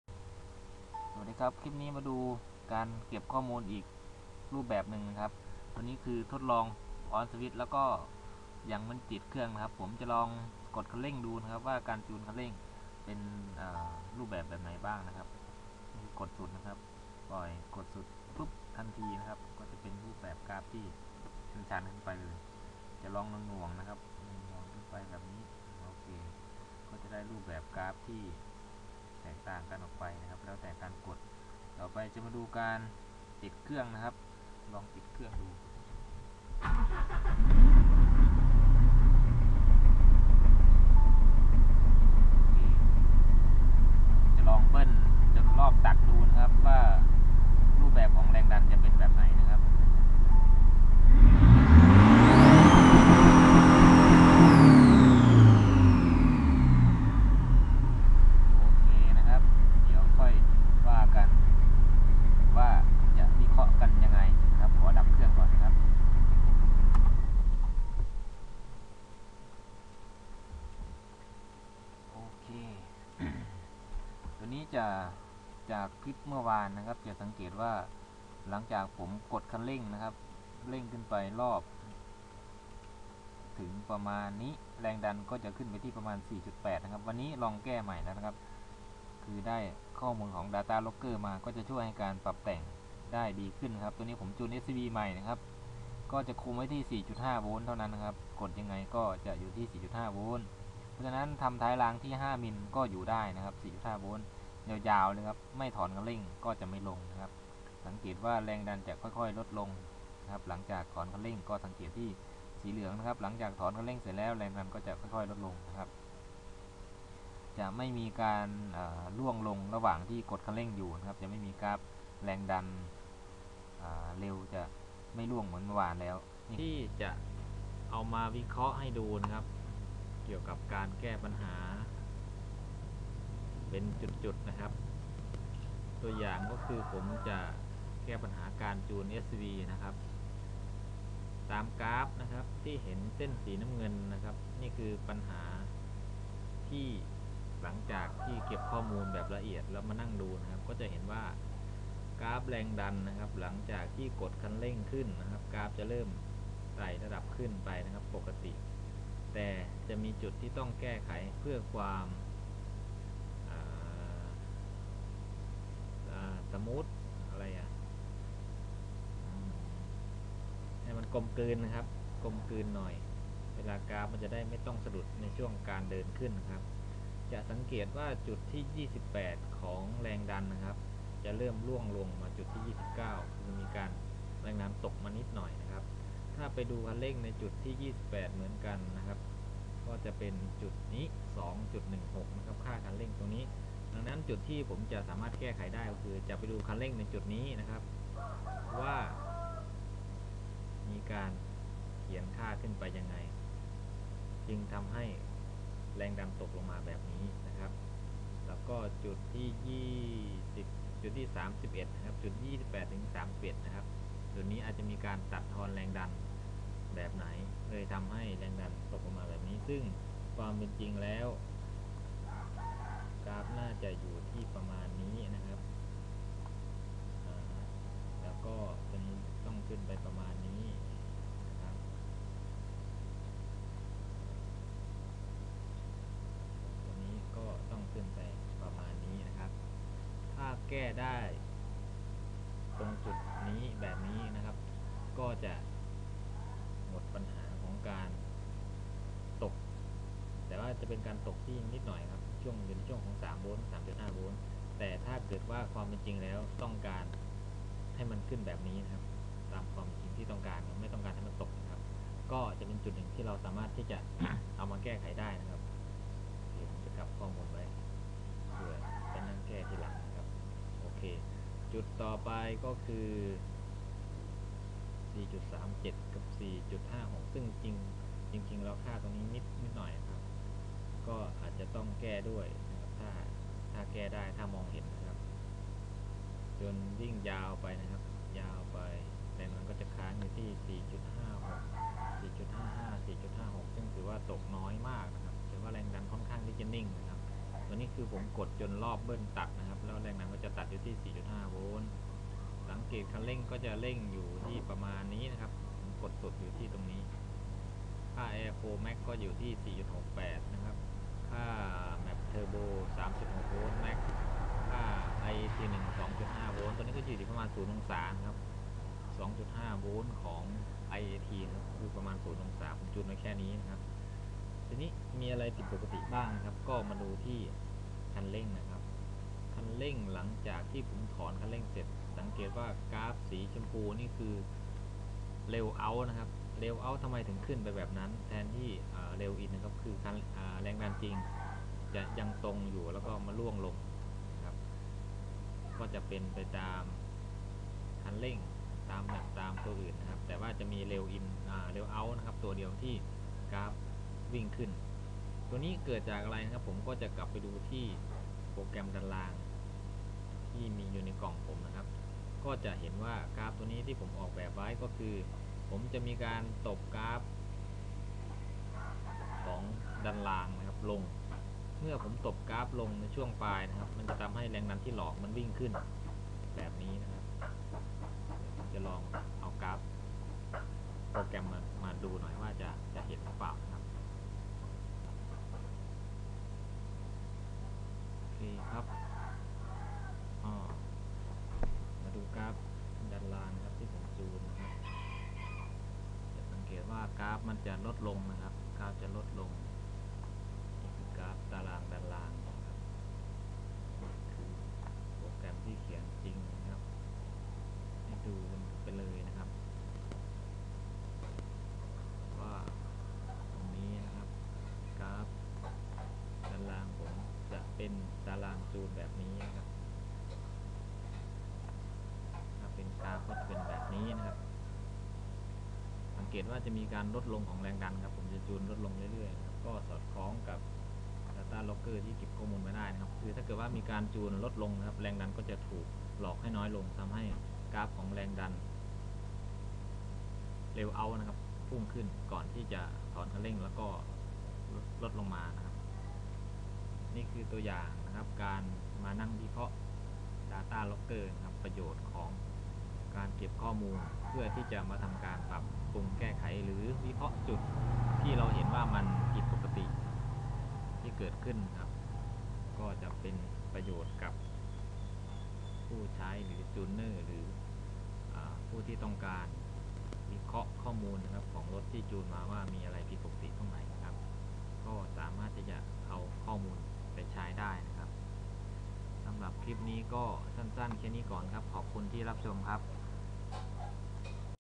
จูนรถ เก็บข้อมูลมาวิเคราะห์ 3
เสนอการนำข้อมูลมาเปรียบเทียบกันแบบละเอียด ทั้งต้นทางและปลายทางให้ดูกันแบบชัดเจน ว่าดาต้าล๊อคเกอร์ มันน่าใช้ขนาดใหน ติดตามผลงาน. บ้าน ๆ .ทางเฟสบุ๊ค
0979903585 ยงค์